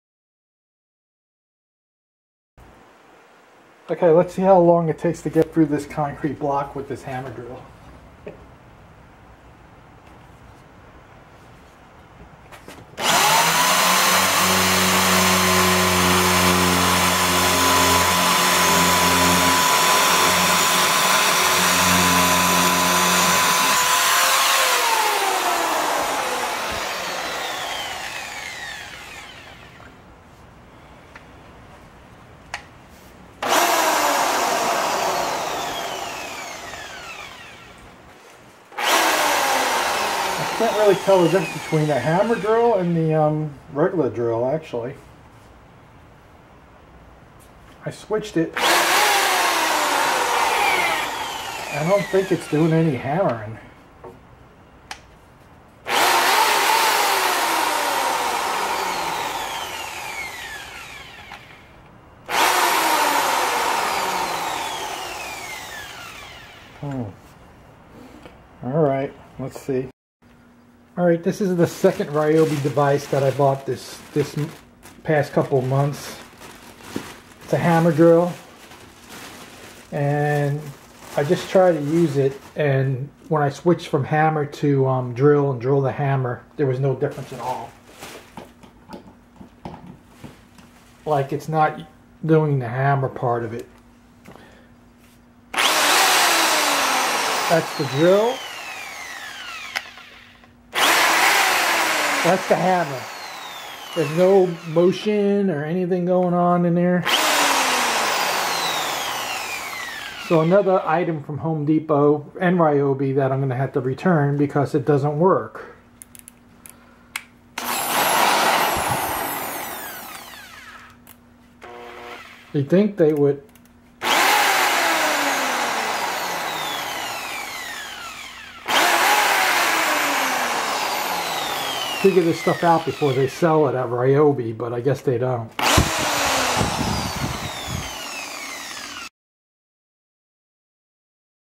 Ryobi Corded Hammer Drill Review D620H did it fail?
I purchased the Ryobi Corded Hammer Drill I had wanted one for a long time to replace many broken other drills in the past. It had to be corded.

Although I believe this drill did not use the hammer potion the drill is cheap. If you need to drill
holes with a larger chuck this may be the way to go. Just don't depend on the hammer portion. Looking for the best hammer drill I don't think this is it. I like that this corded hammer drill works well but the hammer portion has less to be desired.

RYOBI 6.2 Amp Corded 5/8 in. Variable Speed Hammer Drill D620H